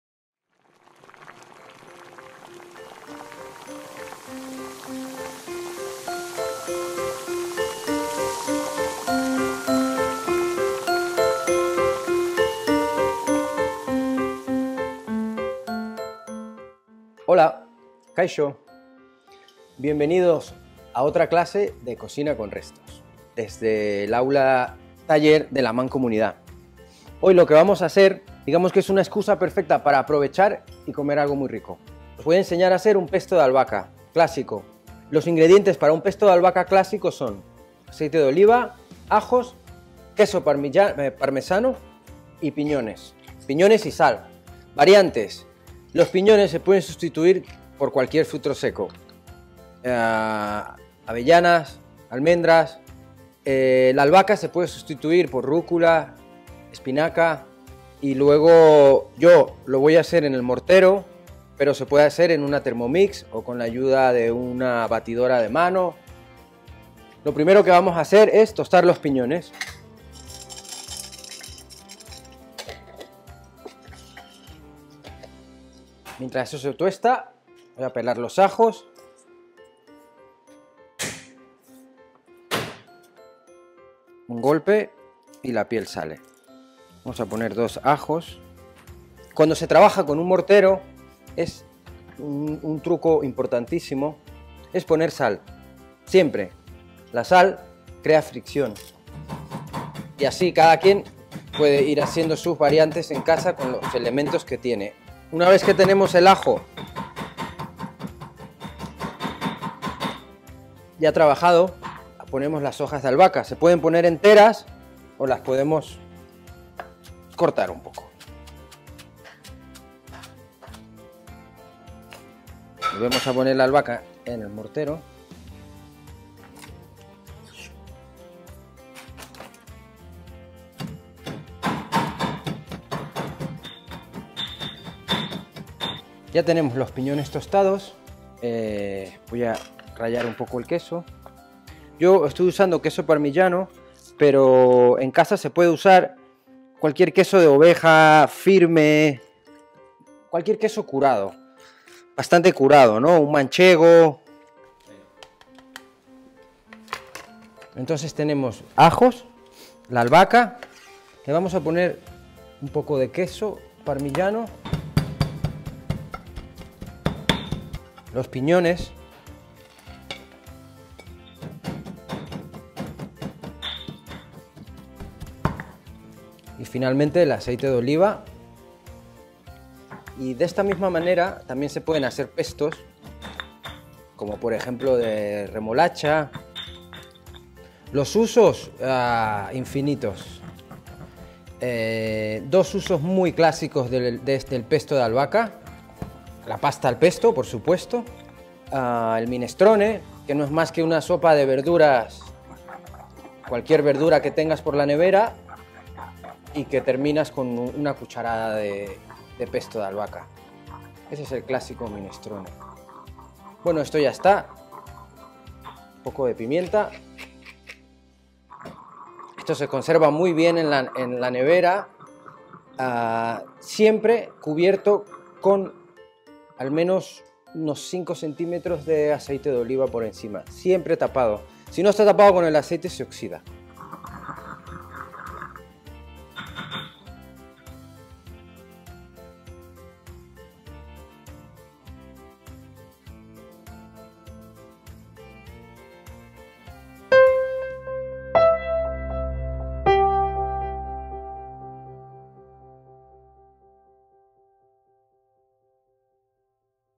CocinaConRestos - Pesto
Nuestro chef Julio Flames nos da ideas super interesantes para aprovechar todos esos restos de comida que nos van quedando en casa. Recetas, trucos, consejos... Bon appetit!